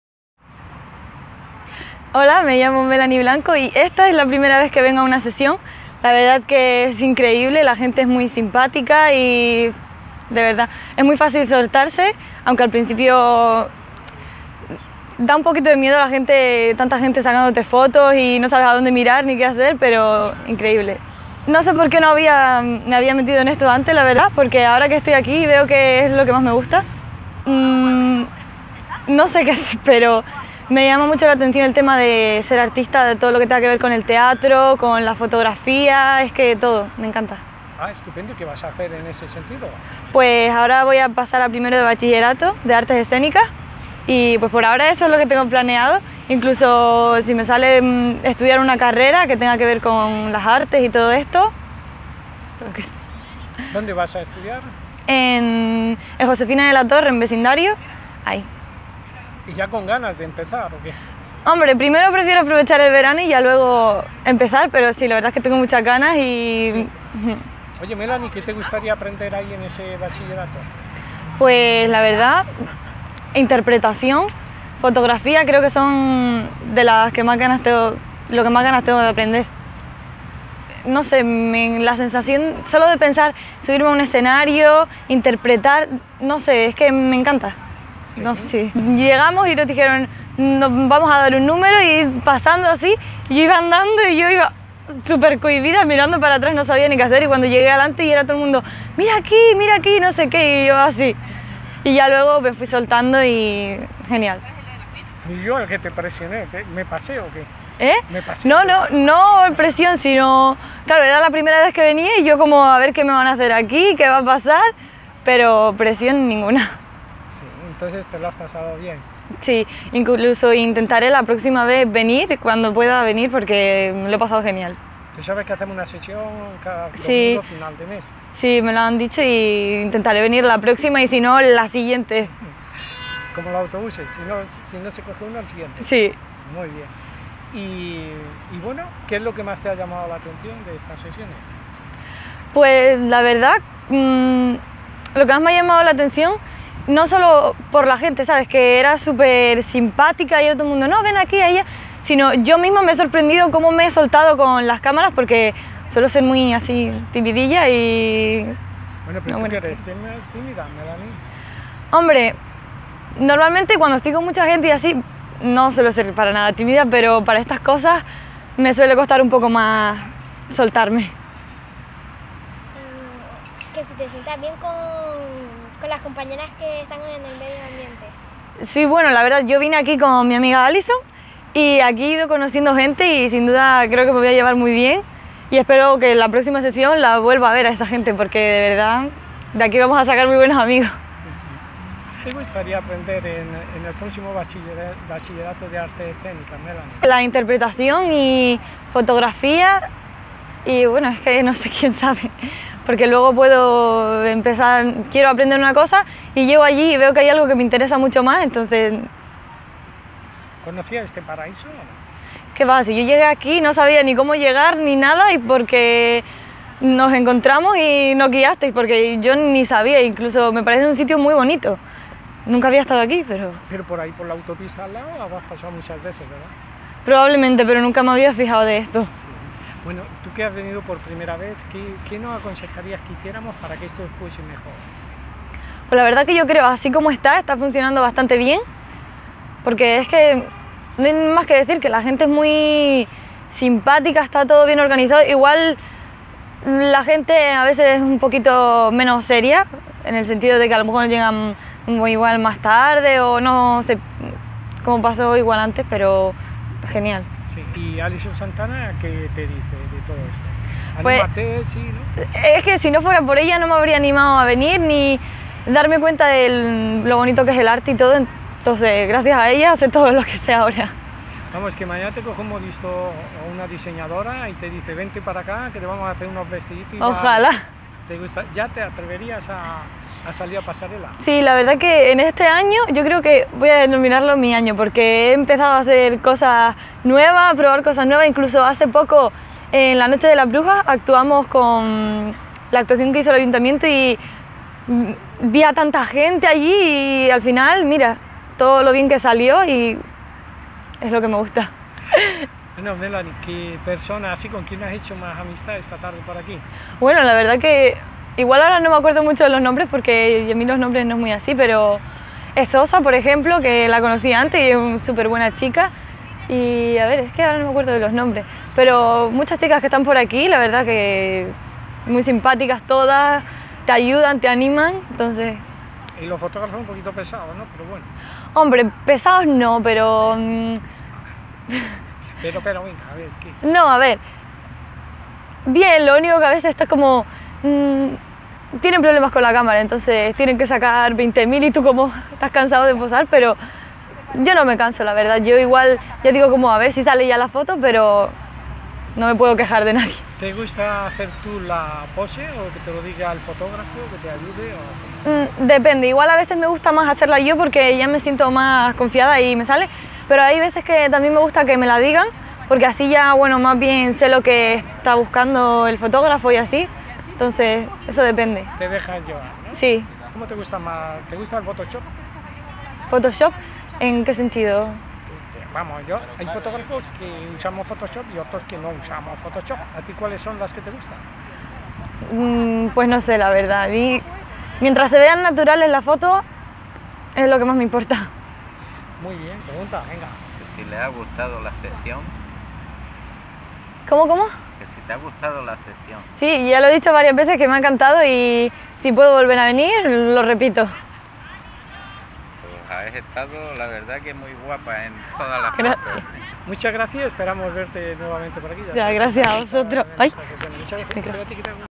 Melani Blanco nos da sus impresiones tras la primera sesión fotográfica fashion.
Las Palmas de Gran Canaria 31 de Julio 2016

Melani Blanco, acude por primera vez en una sesión fotografica  fashion  animada por su amiga Alison Santana, quien si cuenta ya con experiencias  de algunas pasarelas, sesiones fotográficas y trabajar con algunos diseñadores. Melani nos cuenta sus experiencia en esa su primera sesión.